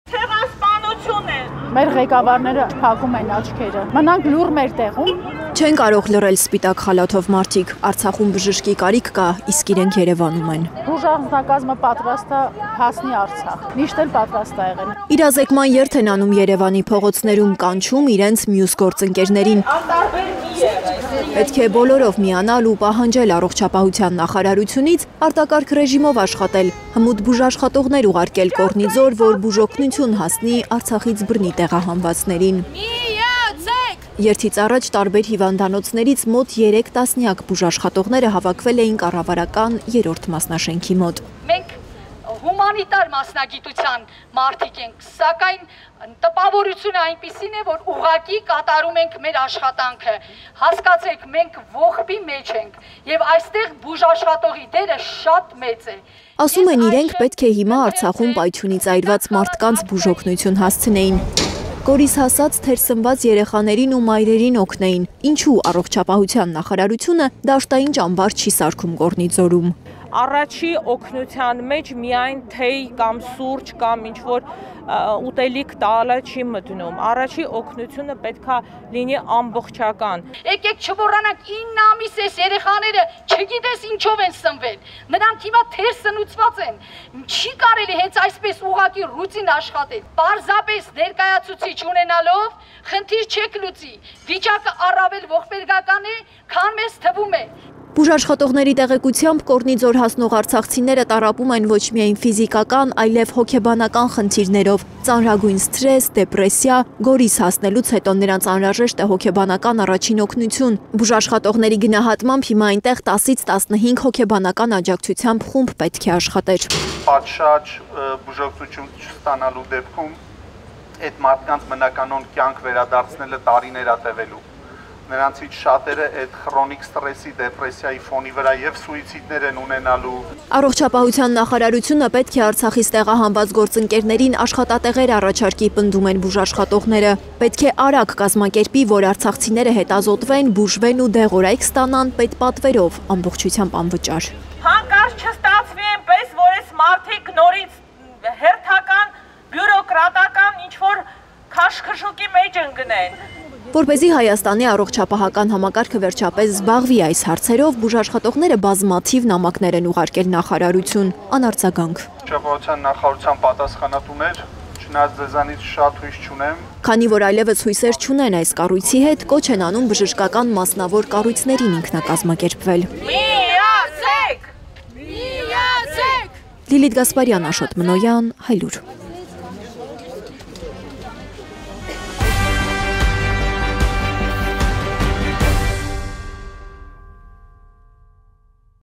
Բուժաշխատողները պատրաստ են Արցախ գնալ և արտակարգ ռեժիմով աշխատանք են պահանջում նախարարությունից
Սպիտակ երթ է եղել այսօր Երևանում: Բուժաշխատողներն են փողոց հանել սպիտակ բանակը, որ պահանջեն առողջապահության նախարարությունից արտակարգ ռեժիմով աշխատելու հրահանգ, որպեսզի կարողանան մեկնել  Կոռնիձոր՝ օգնելու  Արցախից բռնի տեղահանվածներին: